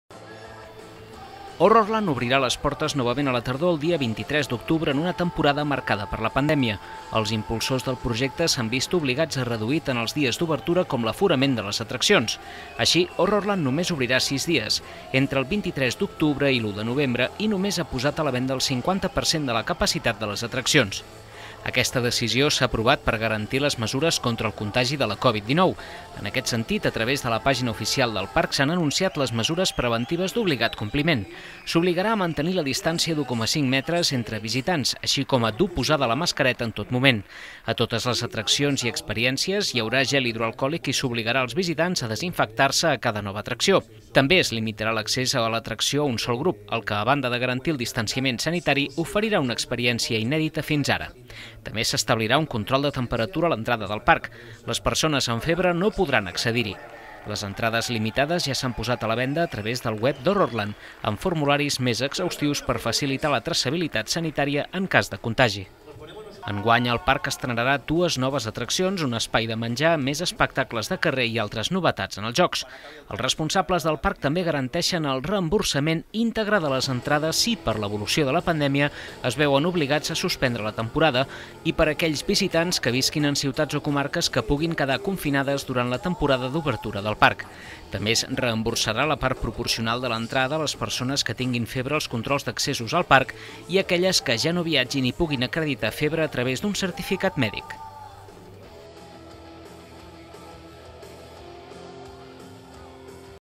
Horrorland reduirà l’aforament a la meitat per salvar la temporada
El parc temàtic de terror Horrorland de Cercs reduirà a la meitat l’aforament de les atraccions aquesta temporada i obrirà menys dies, amb l’objectiu d’evitar contagis i convertir el complex en espai segur. Els impulsors han extremat les mesures sanitàries preventives i de control a través de la venda online d’entrades, ja oberta per el proper Halloween